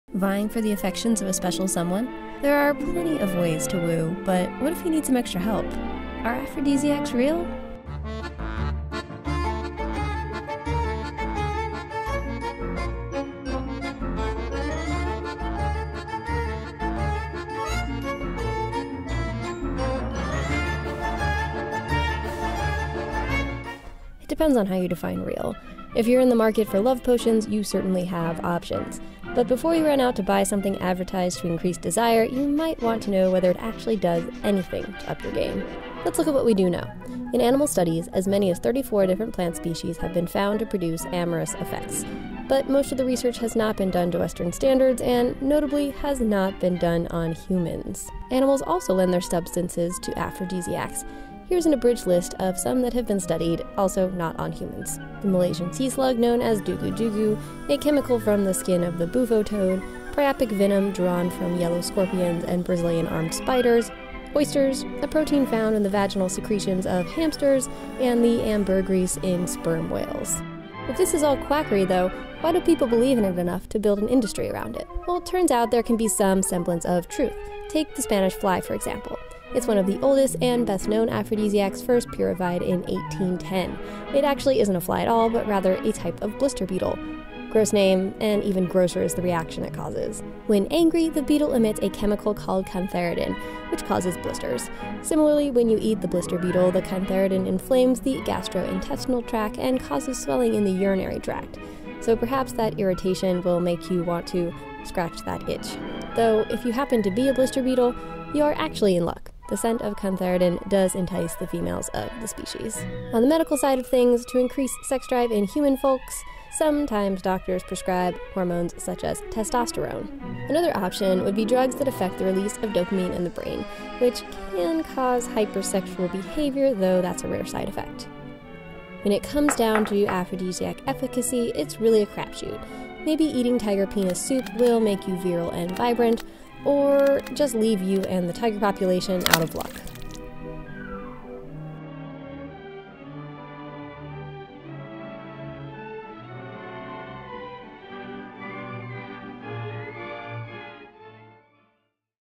Ask Us Anything: Are Aphrodisiacs Real?
Bits of plants and animals are supposed to boost our libido, but what does science say?